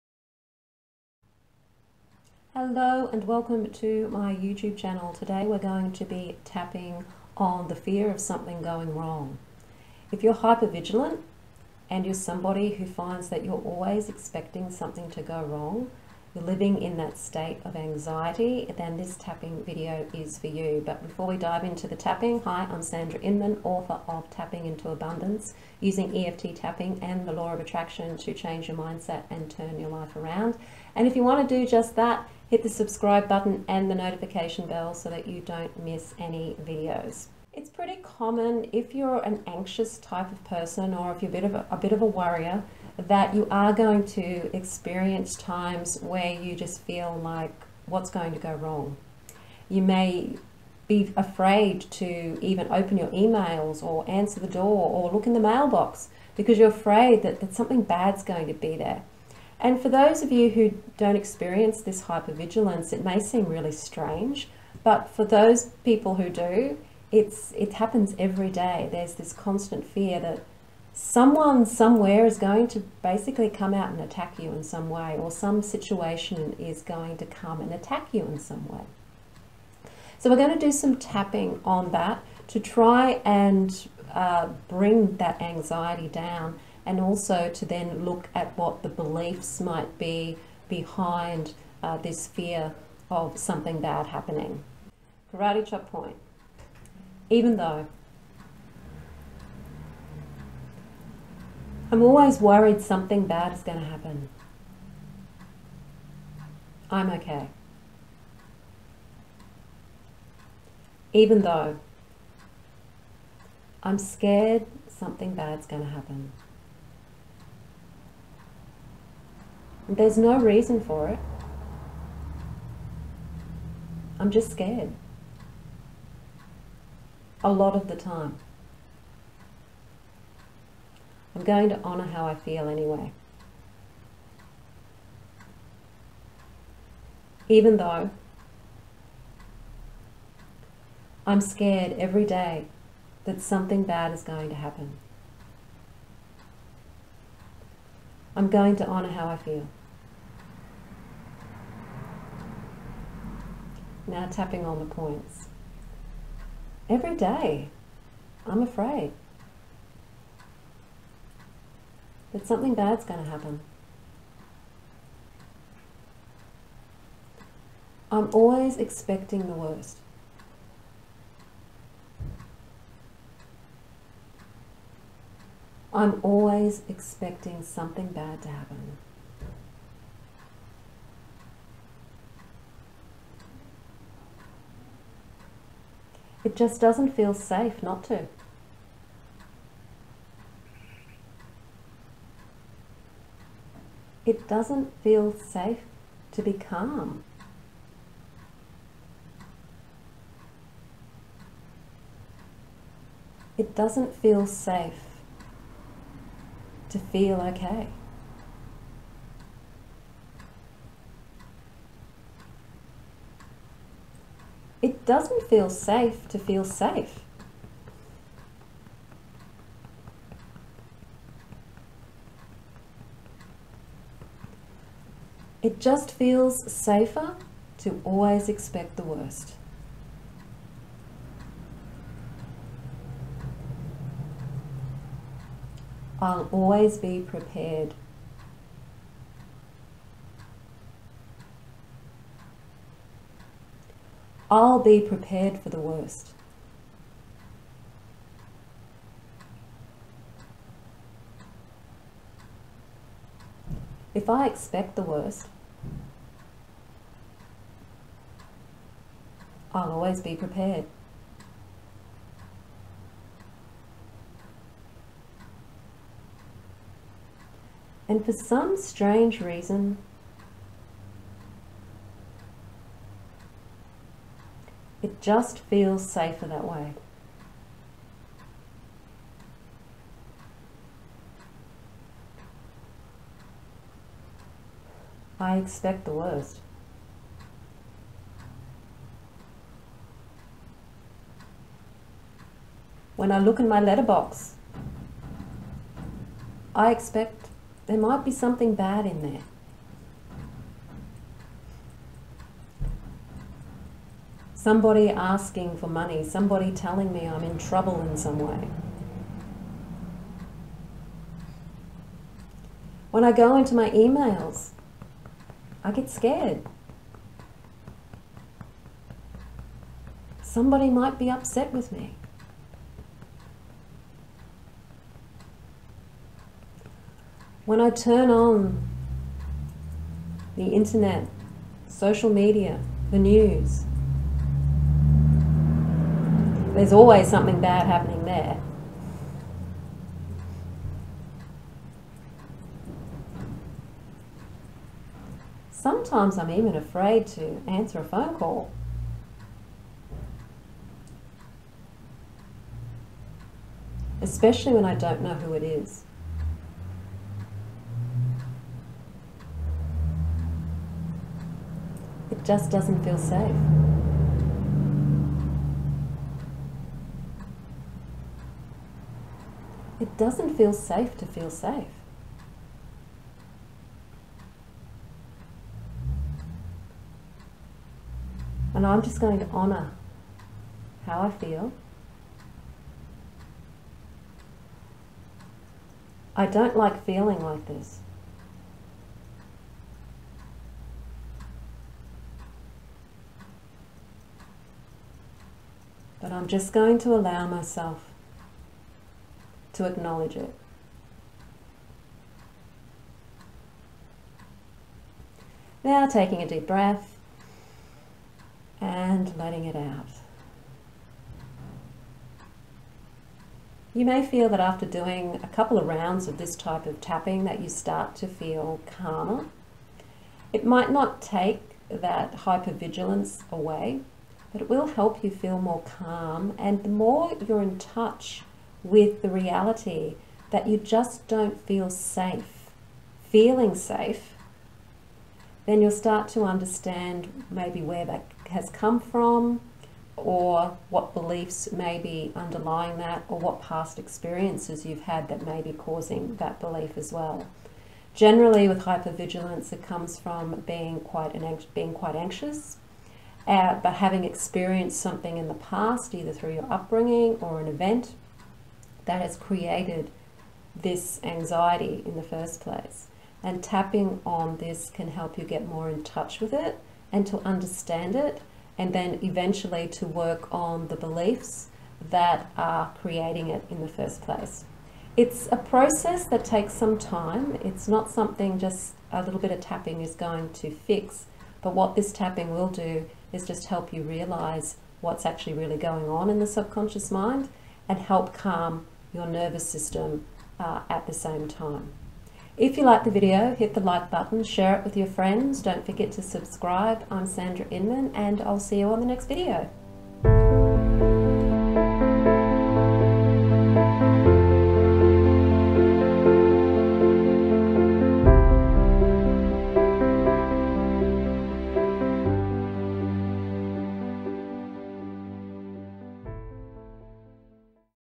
Tapping Away the Fear of Something Bad Happening
If you struggling with hypervigilance or the fear of something bad happening then this tapping may help you calm the nervous system and understand where it's all coming from.

Here's my Book: Tapping Into Abundance - Using EFT Tapping And Law Of Attraction To Change Your Mindset And Turn Your Life Around.